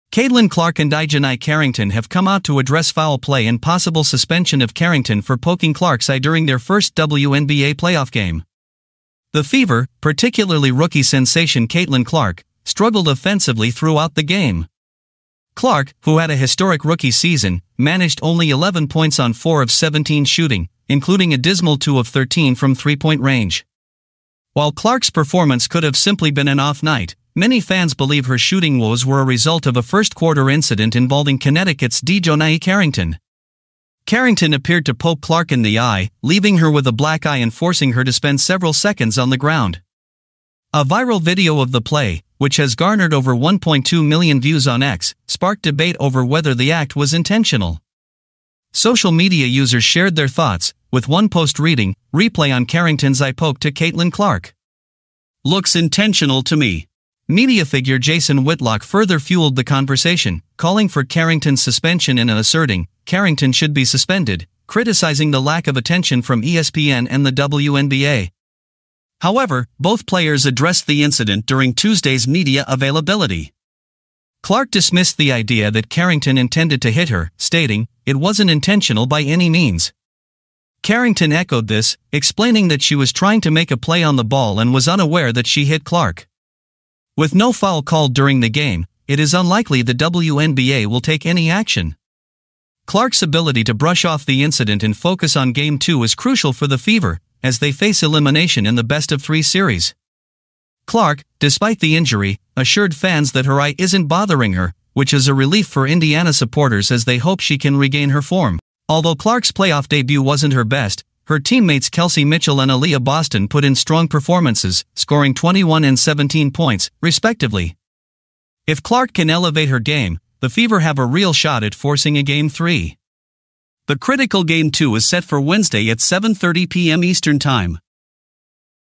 Caitlin Clark and Dijonai Carrington Address SUSPENSION Calls on Carrington in WNBA Playoffs Game
Caitlin Clark and Dijonai Carrington have come out to address calls by WNBA fans to have Carrington Suspended for poking Clark's eye during Indiana Fever vs Connecticut Sun first WNBA Playoffs game. The fans and some media personalities suggested Dijonai Carrington targeted Caitlin Clark's face and poked her eye intentionally. Dijonai Carrington and Caitlin Clark while addressing the media on Tuesday said the incident was not intentional!